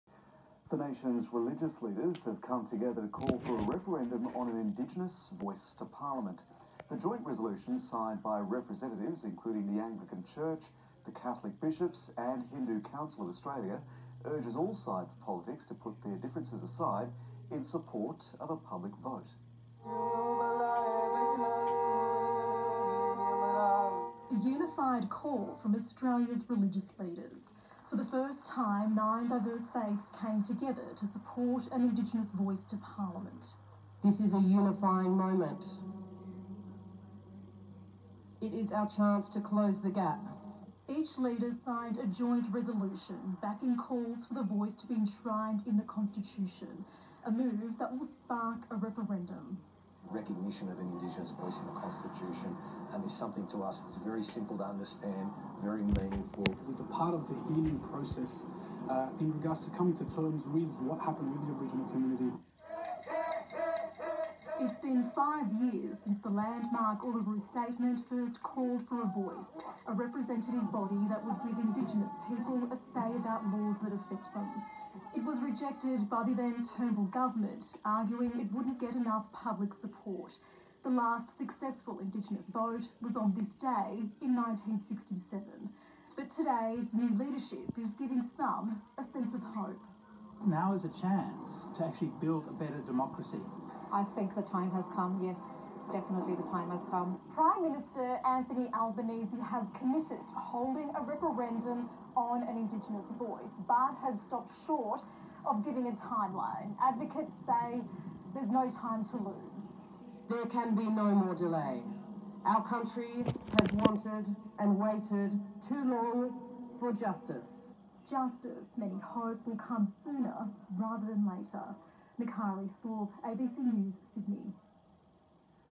Aboriginal’s Show of Faith | Ozkygrouch Video News
Religious leaders from nine faith groups signed a resolution demanding action. The signing marks the 55th anniversary of the 1967 referendum to include First Nations people in the constitution. Anthony Albanese's new federal government has promised to action the statement but has not given a timeline to Uluru.